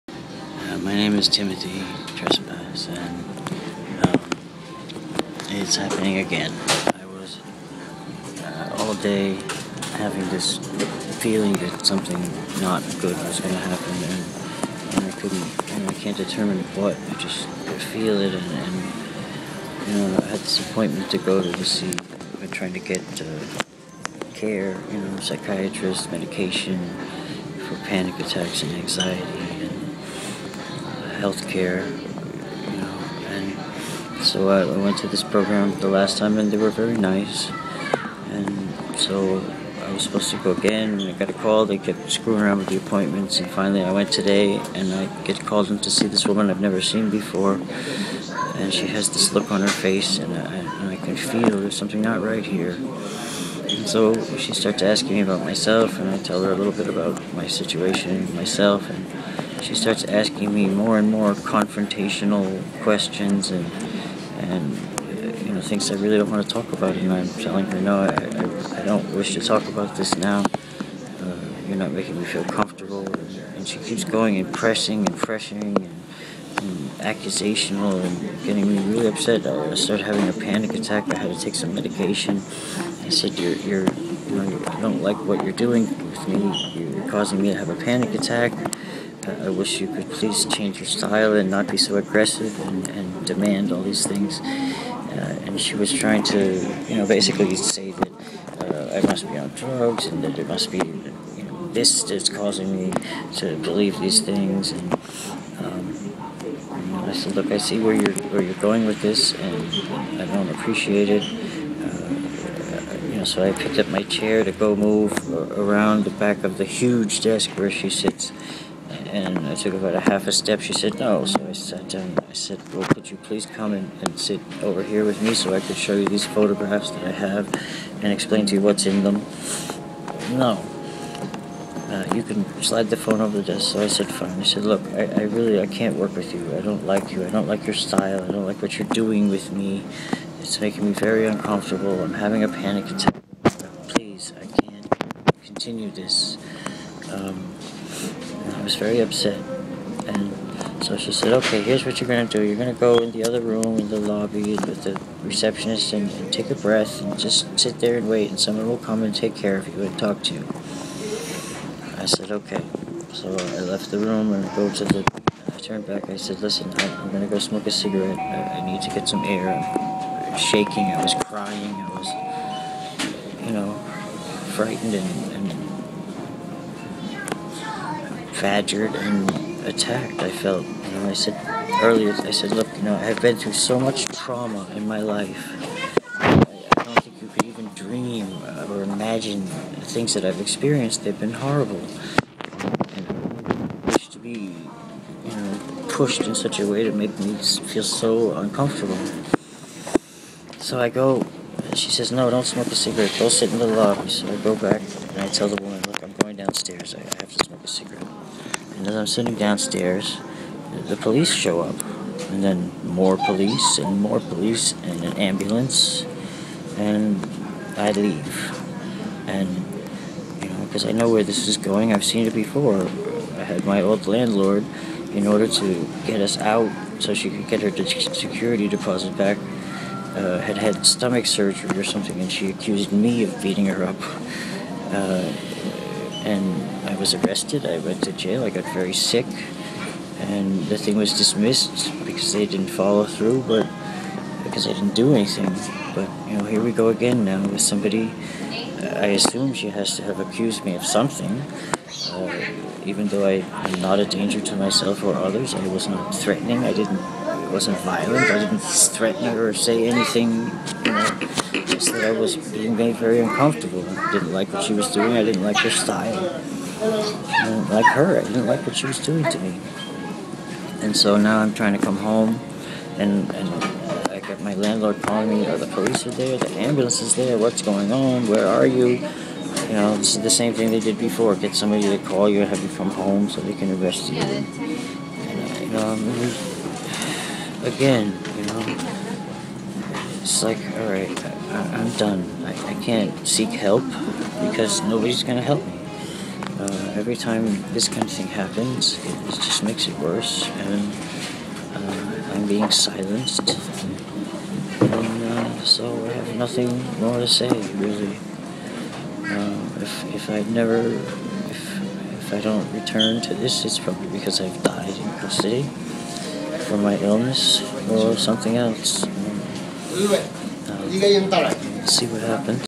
setup? intake CSW called police claiming I was EDP in crisis?
I could not believe it when I fell straight into trap @ 'bridging access to care' intake 4 psychiatrist and md..
please notice: I am not a danger to myself or others, I am physically ill and emotionally traumatized. I went to the BAtC, IN BROOKLYN, a program for people seeking medical and psychiatric assistance, medication, therapy, advocacy, etc. This therapist was acting horribly confrontational and badgering me for incriminating details I wished not to reveal.after accusing me and harassing me to exploit my ptsd  and telling me she wanted me locked in a mental  hospital!  She got me so frustrated i began having a panic attack and frustrated so that she asks me too wait in reception and someone else  Will come talk  with me.i am outside smoking and  police show  up looking the  dangerous emotionally  disturbed Peterson.
when they realize  i left,   they  came too  my  House  and tried  to break  the door down
wow!